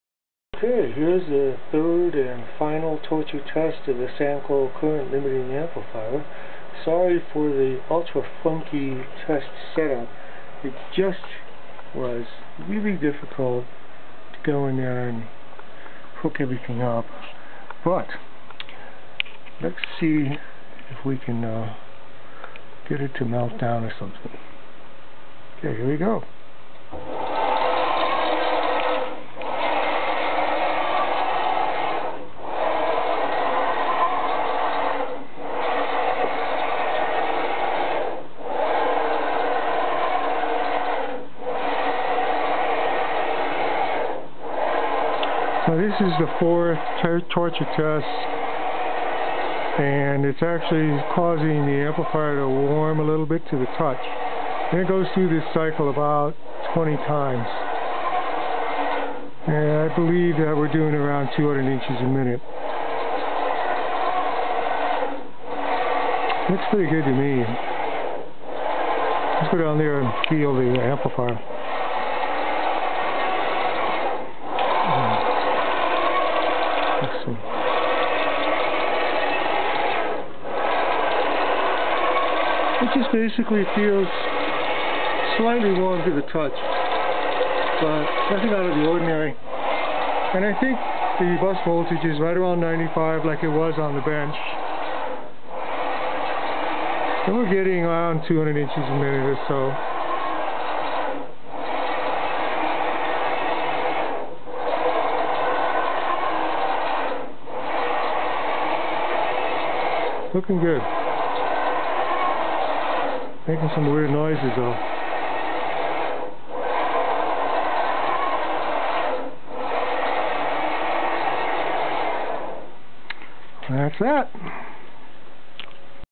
Final test
This is my final torture test for the Samco CL brush type servo amplifier. The original test computer with the Emc2 installation, configuration files, and test programs had an IDE interface failure which ended up taking my whole day to fix. After installing Emc2 on a replacement computer, re-configuring the hal, ini, and PID parameters, writing a new test file, and chasing down 5 or 6 miscellaneous hook up snafus I finally got it to work the way I had envisioned.  I believe that it would be possible to use a bus voltage as high as 150vdc which would give some truly serious rapids and outstanding acceleration for a hydrostatic bearing knee mill.
If I retrofit or build another machine I would like to use this amplifier. I will add an auto emergency bus disconnect, and overtemp shutdown before installing it.  I am very satisfied with the performance of the amplifier.
I am tired, but satisfied.
Tom